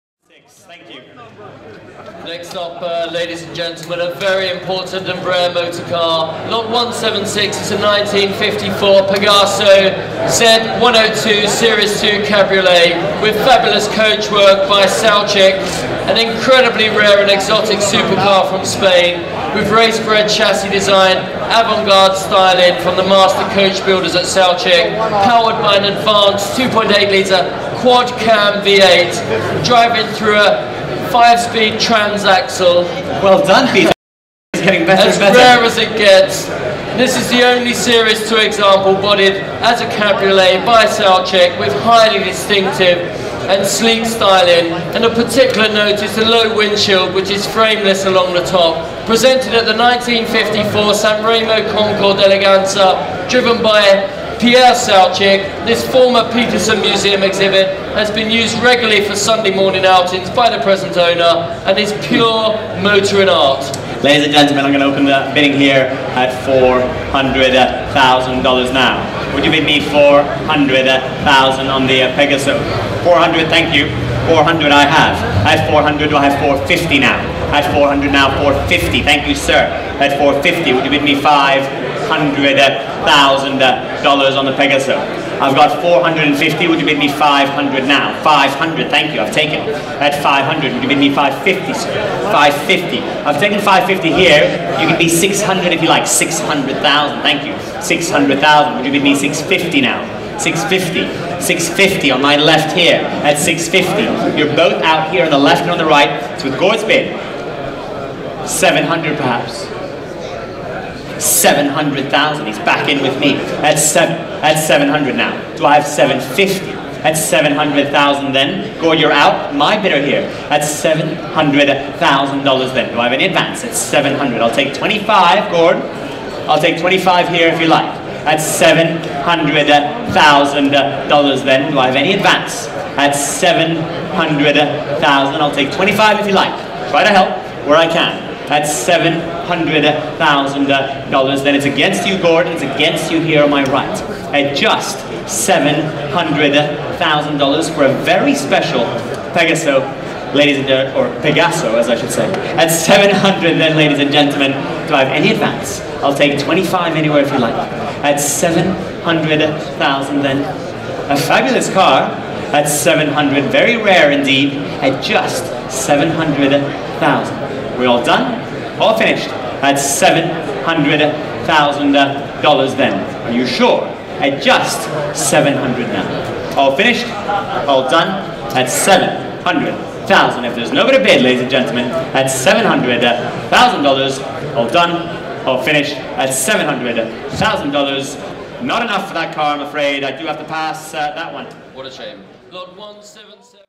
1954 Pegaso Z 102 Series II Cabriolet by Saoutchik
1954 Pegaso Z-102 Series II Cabriolet by Saoutchik
High Bid of $700,000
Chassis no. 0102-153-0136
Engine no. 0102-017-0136
165 bhp, 2,814 cc quad overhead camshaft V-8 engine with a two-barrel Weber carburetor, five-speed manual transaxle, independent front suspension with de Dion-type rear, and four-wheel drum brakes. Wheelbase: 92 in.

•The sole Series II Saoutchik Cabriolet
•A rare example of Spain's modern supercar

While Pegaso was well-established and noted for its production of industrial trucks and coaches, the firm launched what would ultimately be a seven-year foray into the production of a coachbuilt, V-8-powered supercar, the likes of which Spain or the rest of the world would ever see. The chief technical manager for Pegaso was Wifredo Ricart, an experienced designer and engineer whose previous associations included the Alfa Romeo 512, and who was also a rival of Enzo Ferrari, who of course was also an alumnus of Alfa Romeo.

 The Z-102 employed racing-car technology in its chassis and alloy body. All of its components were produced in-house at the Pegaso factory in Barcelona. In the tradition of the grand pre-war classics, the factory produced only rolling chassis that would then be clothed with bespoke coachwork by Touring, Serra, Saoutchik, or Enasa, which was an in-house coachbuilding arm responsible for bodying a number of the chassis in various forms. Power was produced from a four-cam, all-alloy V-8 with dry sump lubrication, which was mated to a five-speed transaxle unit.

 A pair of Z-102 prototypes were built in 1951, one a coupe and one a cabriolet. These examples, which were among those bodied in-house, had steel bodies that lacked the flair sought by Pegaso and inhibited performance due to their weight. Carrozzeria Touring then stepped in and gave the cars a facelift, which involved lowering the bodies, repositioning the fog lights, replacing the grille with a two-piece cross, and revising fine details to produce an overall cleaner profile that was similar to that of the cars it intended to compete with.

 The Z-102 entered production with a 2.5-liter engine, as used in the prototypes, though variants included 2.8- and 3.2-liter engines with multiple aspiration options, including a supercharged 3.2 that produced a whopping 360 horsepower. The fastest examples could reach 160 miles per hour—enough to beat even a production Ferrari at the time—and even the base model had a top speed of 120 mph.

 Like many other luxury and performance machines that had come and gone before the Pegaso, production of these cars spared no expense, which in turn placed a great deal of financial strain on the firm. The later Z-103 was introduced as a simplified, less-expensive version with three different engine offerings, but it did little to improve the company's balance sheet. Production of the Z-102 was discontinued after 1958, with approximately 84 units produced in various combinations of engine, body style, and coachbuilder.
 Chassis 3-0136 is part of what is known as the Second Series by Saoutchik. The Second Series Saoutchik berlinettas and cabriolets were distinguished from each other only by the type of bumpers used. Compared to the earlier First Series, however, they displayed more balanced lines, with less emphatic curvature of the fins, and more space in the wheel arches, and the nose was also revised with a treatment that incorporated the headlights and fog lights inside a teardrop-shaped surround.

 According to the marque reference authored by Carlos Mosquera and Enrique Coma-Cros, there were a total of 18 Saoutchik-bodied Pegasos built; of these, eight were Series II examples, with 3-0136 being the sole cabriolet among them. There were three other Saoutchik cabriolets built; the first, 0-0116, is considered a prototype, and two subsequent examples, 0-0132 and 3-0134, are Series Is. Of particular beauty, and also unique to this car, is the gently curved windshield, which lacks a top frame, leaving only the stylized line of the glass, producing a low, sleek appearance.

 In April of 1954, this car, the last of the Z-102 Saoutchik convertibles, was displayed at the San Remo Concours d'Elegance in a striking blue, driven by Jacques Saoutchik's son, Pierre, who had by then taken over the family business. The car was then sold into private hands, and by February 1958, its owner decided to have it converted into a berlinetta.